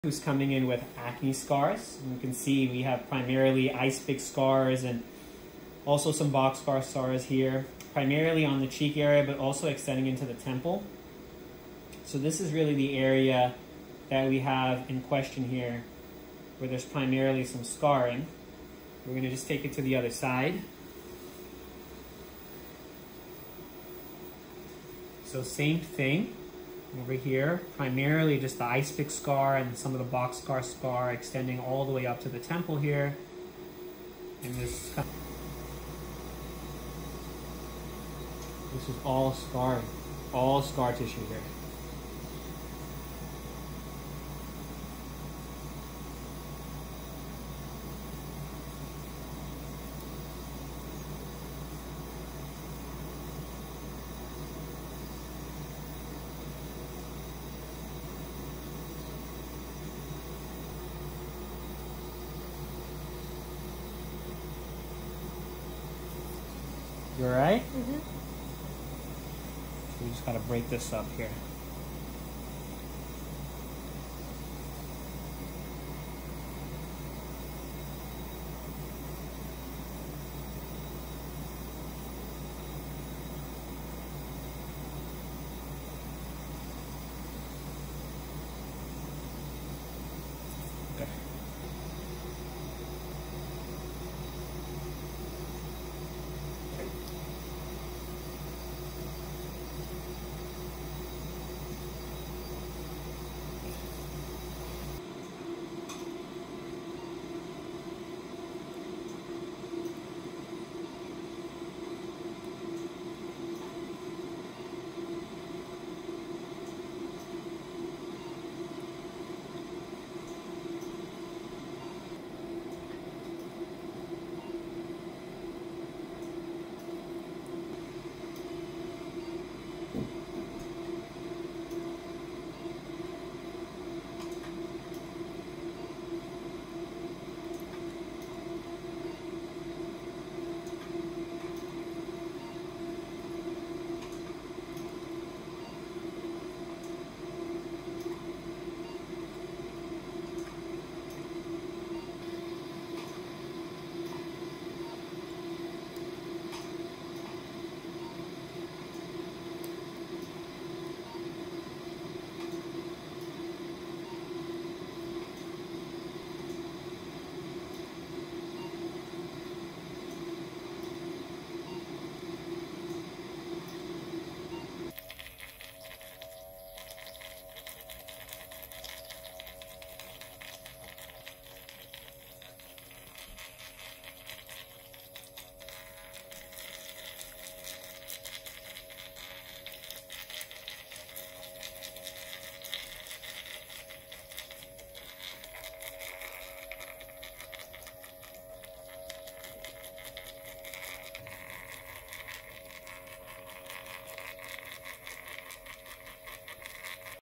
Radio frequency (RF) NEEDLING for Acne Scars with collagen builders and subcision
I am using Radio frequency (SECRET RF) Needling on this young woman with acne scars. This procedure will have about 2 days of downtime and can be performed in skin of ANY color without risk of hyper or hypopigmentation. Several sessions will be needed and in this case I am combining with collagen builders and subcision for a comprehensive approach.